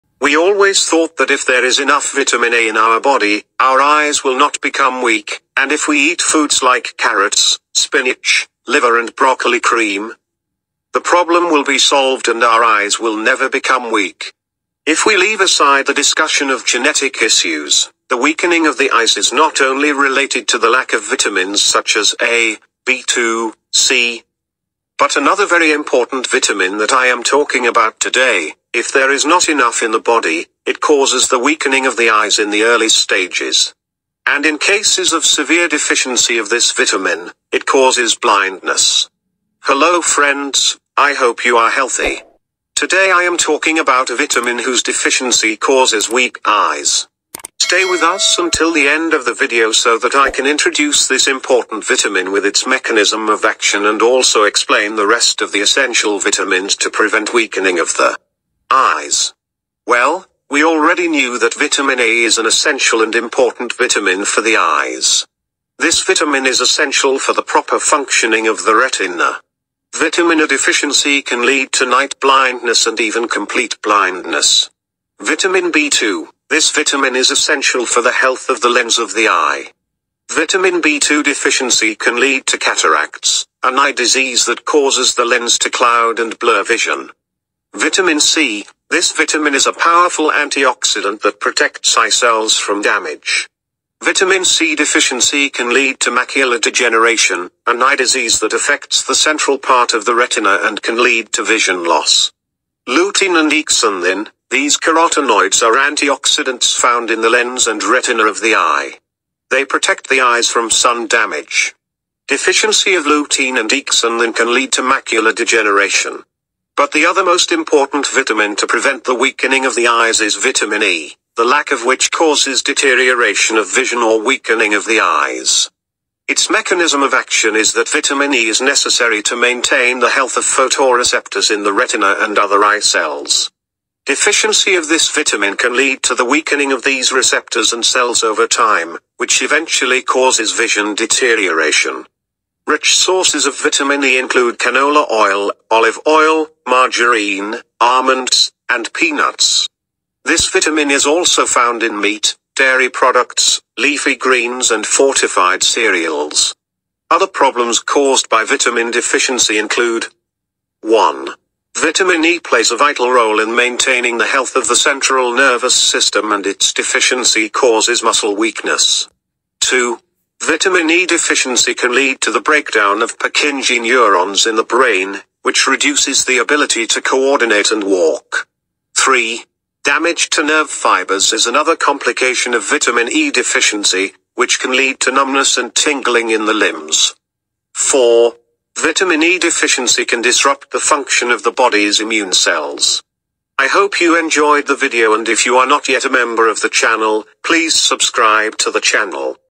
Prevent weakening of the eyes by taking this vitamin!!
We always thought that if there is enough vitamin A in our body, our eyes will not become weak, and if we eat foods like carrots, spinach, liver and broccoli cream, the problem will be solved and our eyes will never become weak.